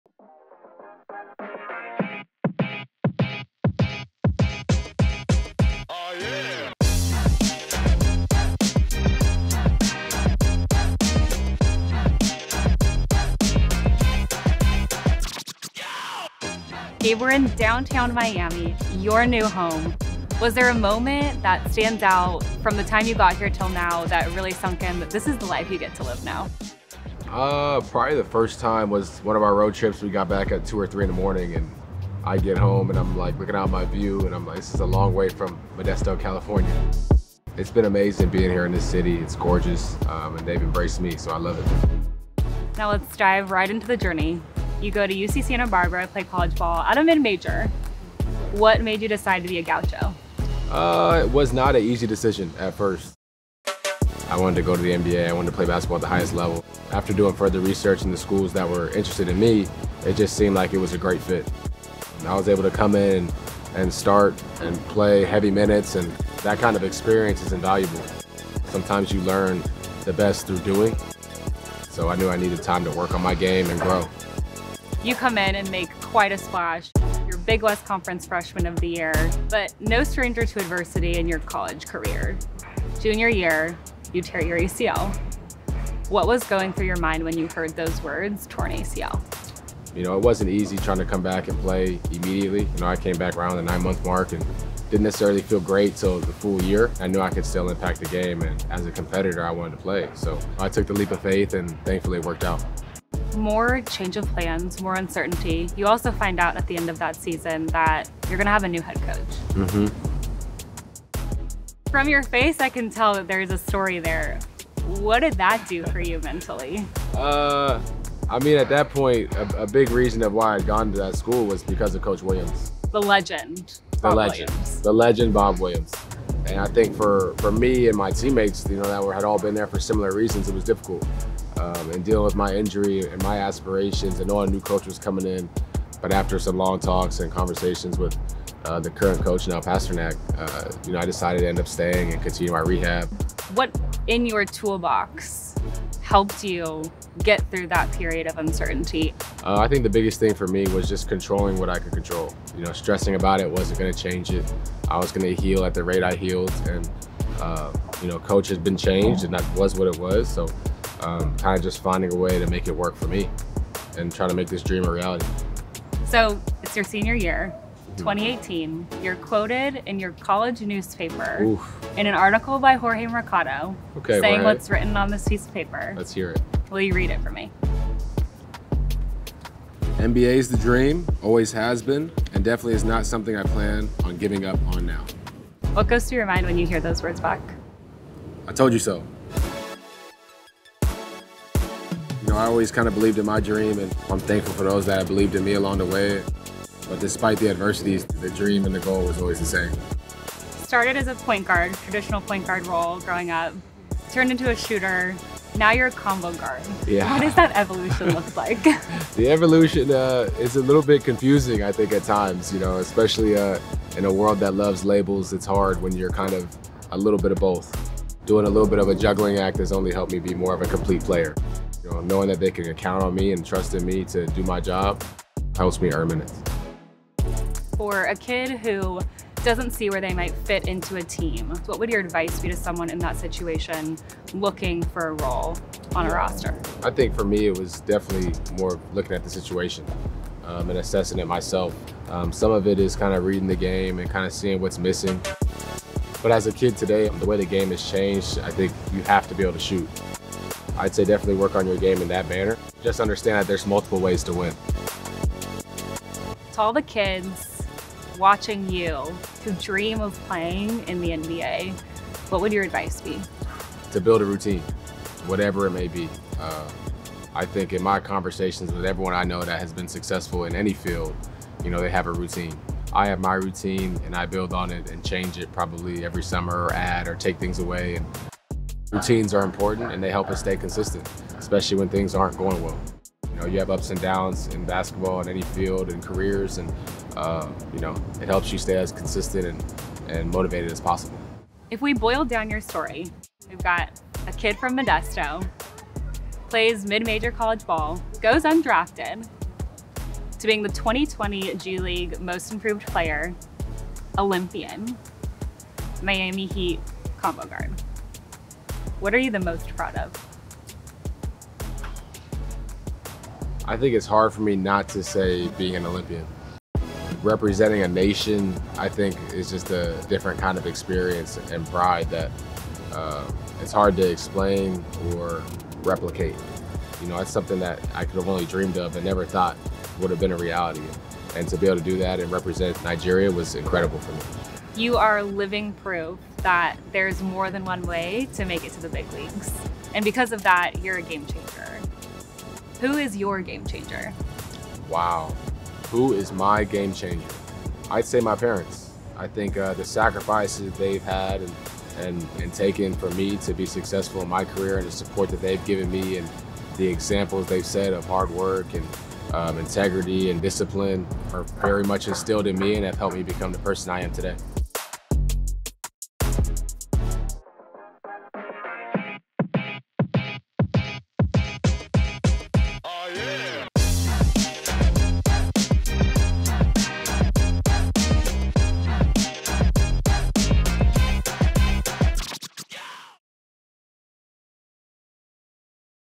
Gabe Vincent | GameChanger Stories Ep. 6
Gabe Vincent of the Miami Heat is a GameChanger. From playing at a mid-major and going undrafted, to starting for the Heat, Gabe proved that with hard work and dedication anything is possible. Tune in to our next episode of GameChanger Stories to hear from the point guard on what it takes to earn starting minutes, how to make yourself invaluable and how to succeed in the face of adversity.